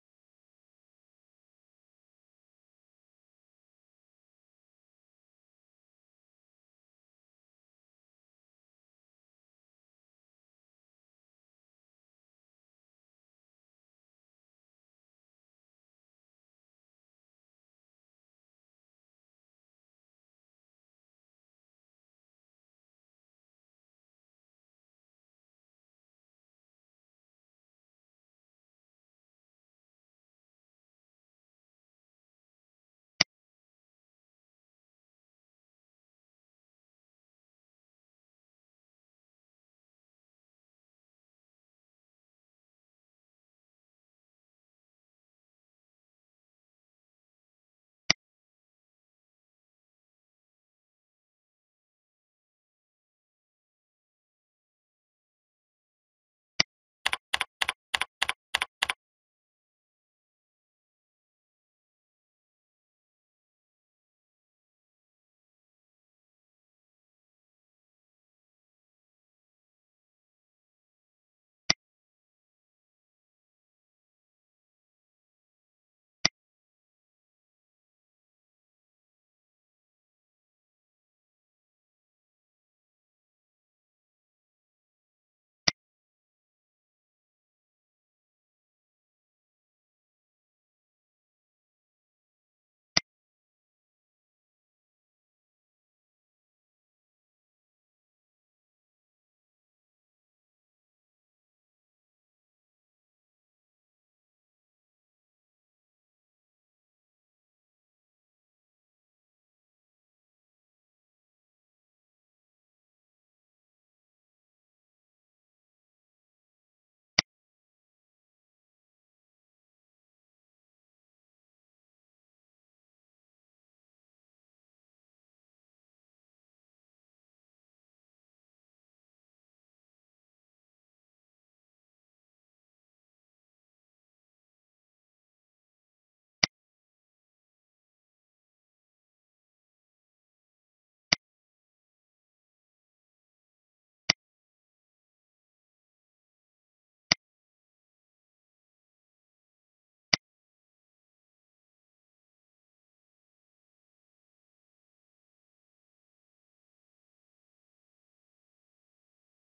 La correction automatique dans Word - Formation Word Marseille
Tutoriel vidéo sur la fonctionnalité de Microsoft Word : Correction automatique
issu de la formation Word Base de Optivalys Formations à Marseille.